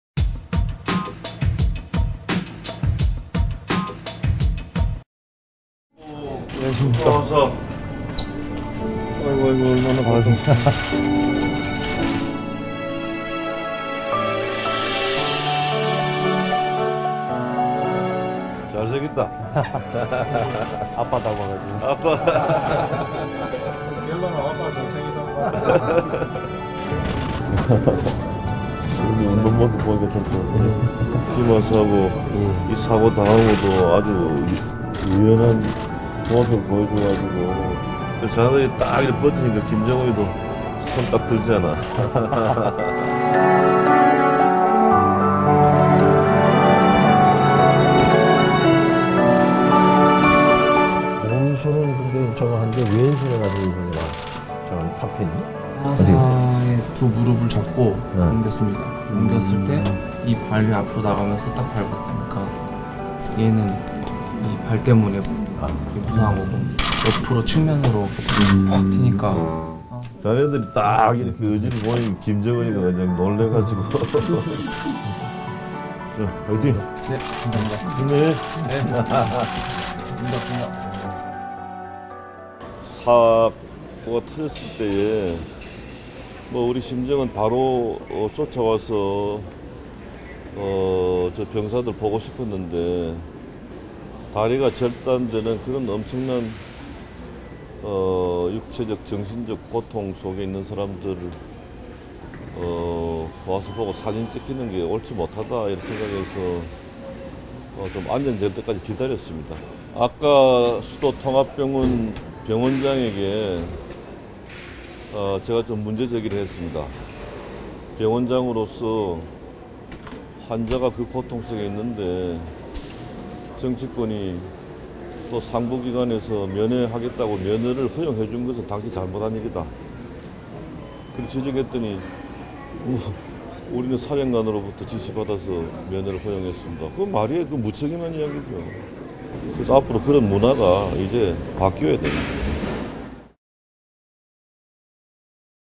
[뒤카] 김무성의 '병문안 문화'(?)
지난달 31일 북한 목함지뢰도발 피해병사들에게 병문안을 간 김무성 새누리당 대표. 김 대표는 병사들의 손을 잡으며 "웃는 모습을 보니 보기가 좋다"며 "의연한 모습을 보여줬고, 자네들이 딱 버티니 김정은도 두손을 딱 들었다"고 말했다.

그런데 병문안을 마치고 나온 후 김 대표는 "사고가 터졌을 때 바로 와서 병사들을 보고 싶었는데, 다리가 절단되는 그런 엄청난 육체적 정신적 고통에 있는 사람들을 와서 사진찍는 게 옳지 못하다 생각해 기다렸다"고 돌연 입장을 밝혔다. 이어 김 대표는 "아까 수도통합병원장에게 문제제기를 했다"며 "병원장으로서 환자가 그 고통속에 있는데... 정치권이, 또 상부기관에서 면회를 하겠다고 허용해준 것은 잘못한 일이라 지적했다"고 말했다. 김 대표의 지적에 수도통합병원장은 '사령관으로부터 지시를 받아 면회를 허용했다'고 답했다는데... 김 대표는 이에 대해 "그게 말인가? 무책임한 얘기지"라고 평가했다. 김무성 대표의 '병 문안 문화(?)'에 대한 생각을 '뒤카'에 담았다.

영상편집=뒤카제작단